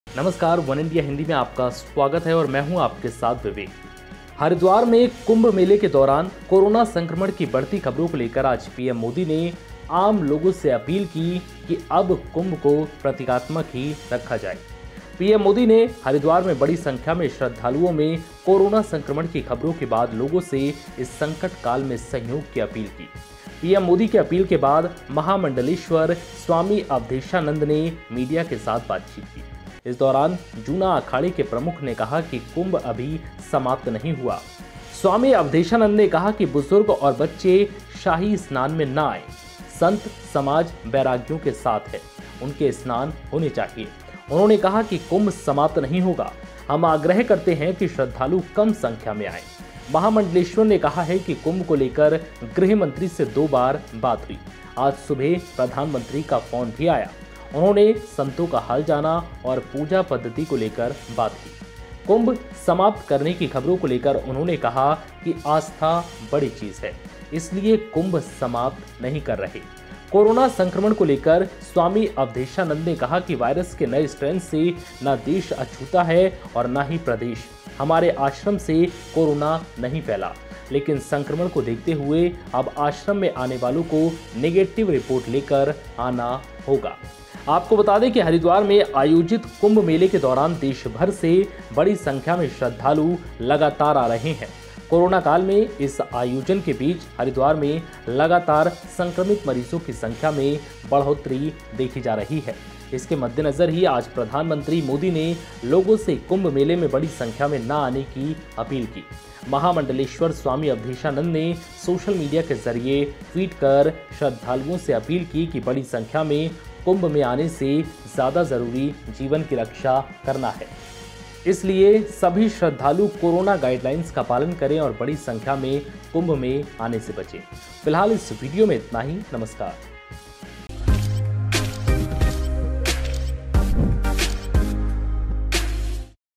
Kumbh: PM Modi की अपील के बाद बोले Avdheshanand- कुंभ समाप्त नहीं हुआ | वनइंडिया हिंदी
Swami Avdheshanand in an interview with a new channel said that Covid protocols are being maintained in Kumbh. He spoke about the conversation with Prime Minister Narendra Modi. Taking to Twitter, the Prime Minister said that he spoke to top seer. Hindu Dharma Acharya Sabha president Swami Avdheshanand Giri Ji Maharaj over phone and made an appeal about the event.

उत्तराखंड के हरिद्वार में कुंभ मेले के दौरान कोरोना संक्रमण की बढ़ती खबरों को लेकर आज पीएम मोदी ने आम लोगों से अपील की कि अब कुंभ को प्रतीकात्मक ही रखा जाए. पीएम मोदी ने हरिद्वार में बड़ी संख्या में श्रद्धालुओं में कोरोना संक्रमण की खबरों के बाद लोगों से इस संकट काल में सहयोग की अपील की. पीएम मोदी की अपील के बाद महामंडलेश्वर स्वामी अवधेशानंद ने मीडिया के साथ बातचीत की. देखें वीडियो